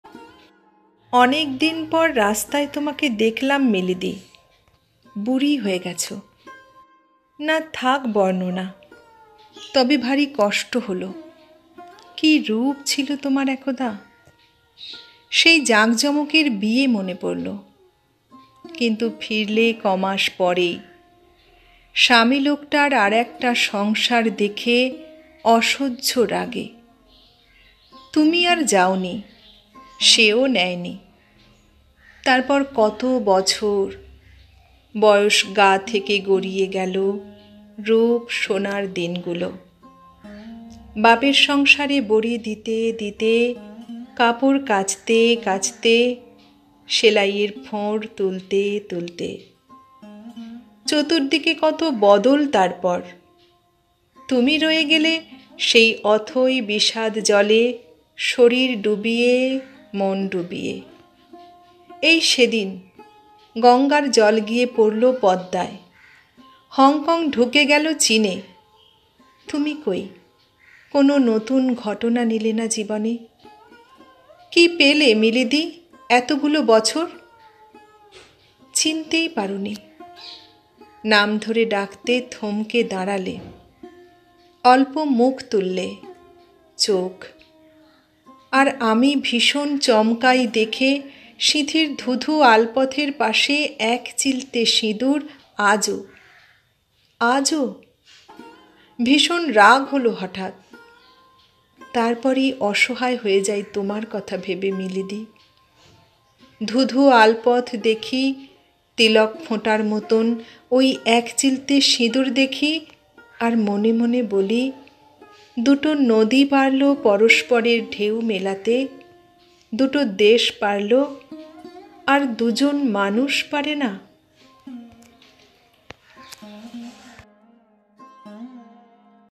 মিলি দি ।ব্রত চক্রবর্তী ।Mili di Brata Chakrabarty ।bangla kobita abritti ।Tandra Sarkar
Poem- Mili di।মিলি দি
Poet- Brata Chakrabarty ।ব্রত চক্রবর্তী
Recitation- Tandra Sarkar

brata chakrabarty kobita mili di
mili di kobita
মিলি দি কবিতা ব্রত চক্রবর্তী
ব্রত চক্রবর্তী রচিত কবিতা মিলি দি